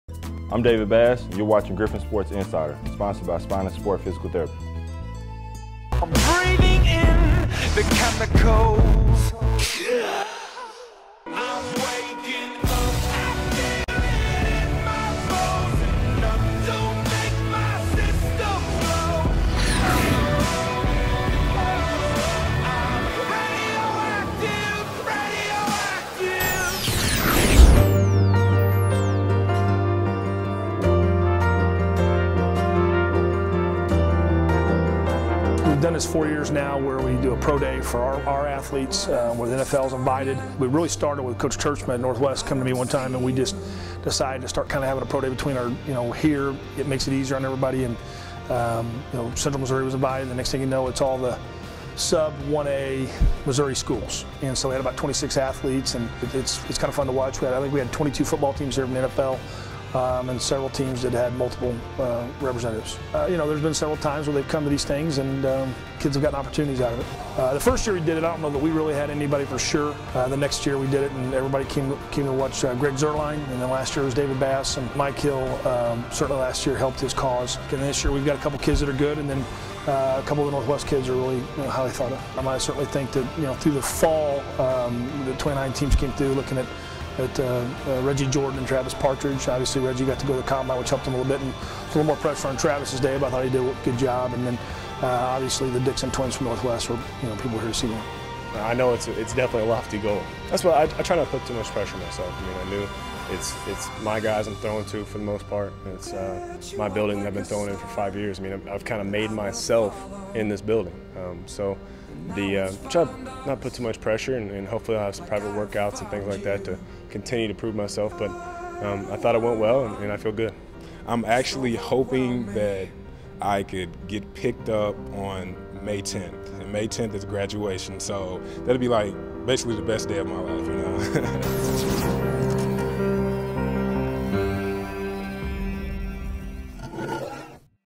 NFL Hopefuls Showcased at MWSU Pro Day
Former Griffon football players Reggie Jordan, Travis Partridge and 24 other college football players from around the "Show-Me State" showed their stuff for scouts from 22 NFL teams at Missouri Western's 4th Annual Pro Day Friday.